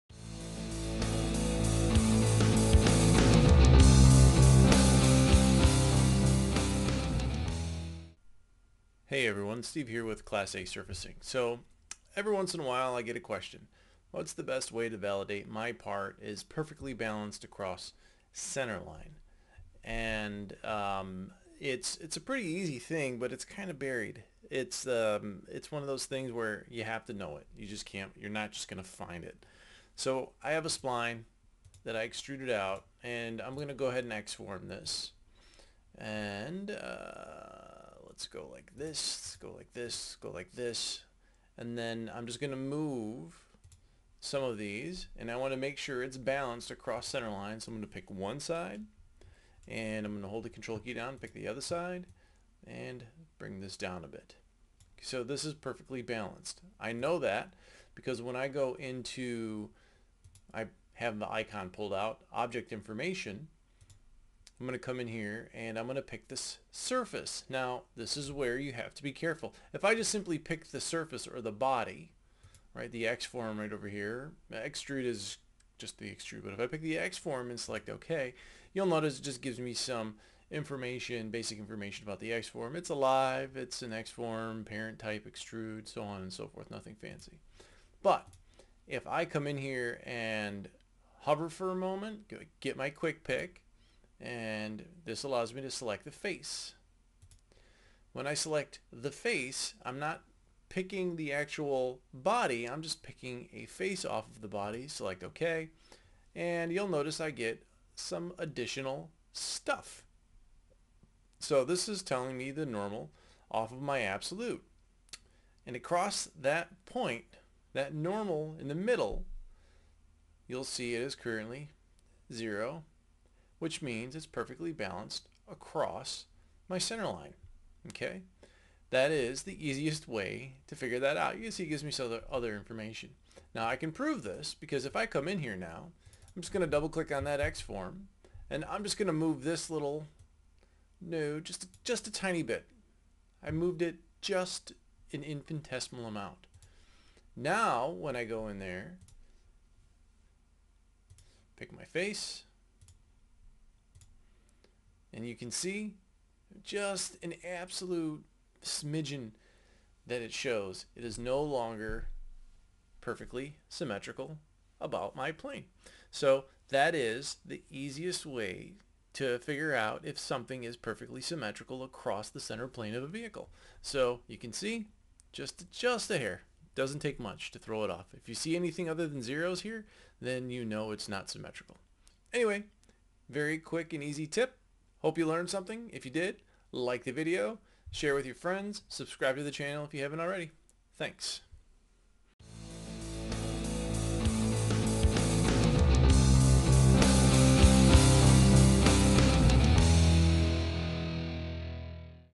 Siemens NX: Perfect Balance
How to verify your surfaces are perfectly balanced across the center line of a vehicle.